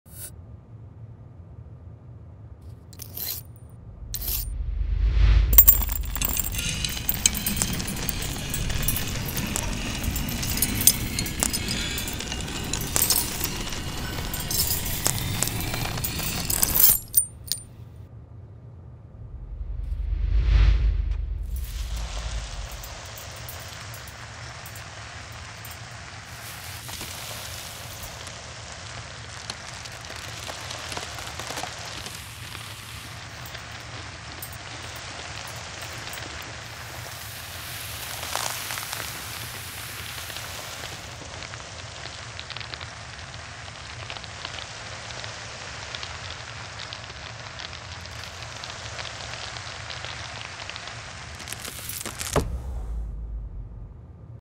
Why You Should Wash Knife After Sharpening
Cutting Cheese After Sharpening a Knife in Macro. ASMR.
Further more ASMR macro videos!